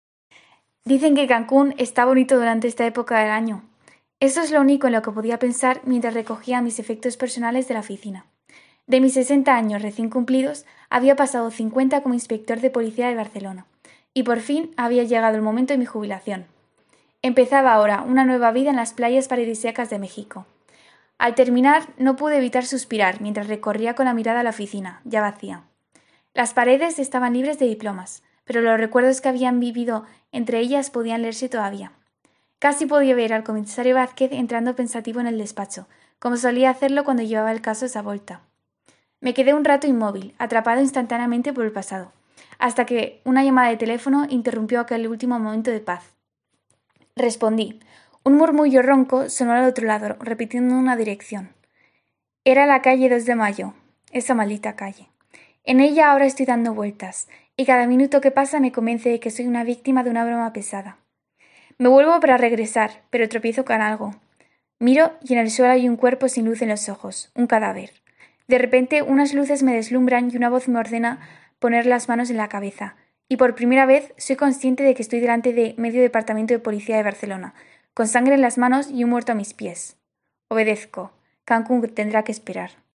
Colegio Santa María La Blanca | 2º BACHILLERATO | 2ºBACH A | Almudena Ramos Platero
Centro Educativo:
Colegio Santa María La Blanca
Curso:
2º BACHILLERATO
Aula:
2ºBACH A
Nombre del Alumno/a:
Almudena Ramos Platero
Nombre del Profesor/a:
Clara Díaz Diéguez

Microrrelato:
"Eso es lo único en lo que podía pensar mientras recogía mis efectos personales de la oficina. De mis sesenta años recién cumplidos, había pasado cincuenta como inspector de policía de Barcelona, y por fin había llegado el momento de mi jubilación. Empezaba ahora una nueva vida en las playas paradisíacas de México.
Al terminar no pude evitar suspirar mientras recorría con la mirada la oficina, ya vacía. Las paredes estaban libres de diplomas, pero los recuerdos que había vivido entre ellas podían leerse todavía.
Casi podía ver al comisario Vázquez entrando pensativo en el despacho, como solía hacerlo cuando llevaba el caso Savolta.
Me quedé un rato inmóvil, atrapado instantáneamente por el pasado, hasta que una llamada teléfono interrumpió aquel último momento de paz.
Respondí. Un murmullo ronco sonó al otro lado, repitiendo una dirección. Era en la calle Dos de Mayo. Esa maldita calle.
En ella estoy dando vueltas ahora, y cada minuto que pasa me convence de que soy víctima de una broma pesada. Me vuelvo para regresar y tropiezo con algo. Miro, y en el suelo hay un cuerpo sin luz en los ojos. Un cadáver. De repente unas luces me deslumbran y una voz me ordena poner las manos en la cabeza. Y por primera vez soy consciente de que estoy delante de medio departamento de policía de Barcelona con sangre en las manos y un muerto a mis pies.
Obedezco. Cancún tendrá que esperar."

Obra de referencia:
El microrrelato está relacionado con la obra de "La verdad sobre el caso Savolta" de Eduardo Mendoza. El narrador es uno de los comisarios que trabajaron con el comisario Vázquez en el caso.